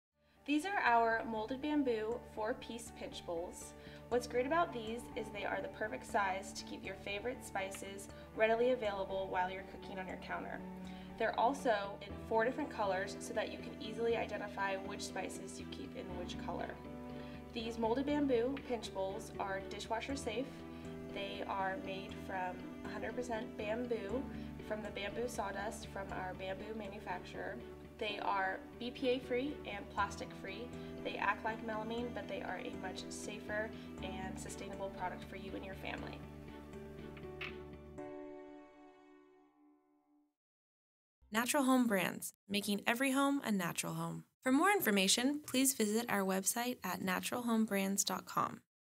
Molded Bamboo® Pinch Bowls
Natural Home Molded Bamboo®  Pinch Bowls can be used to measure, stage, pinch and pour ingredients. Conveniently serves sauces, dips and salsa as well! These bowls are made out of durable molded bamboo providing you with a chemical free cooking and baking experience. Sleek, stackable, and space-saving, use Molded Bamboo® pinch bowls to help you get more organized in the kitchen.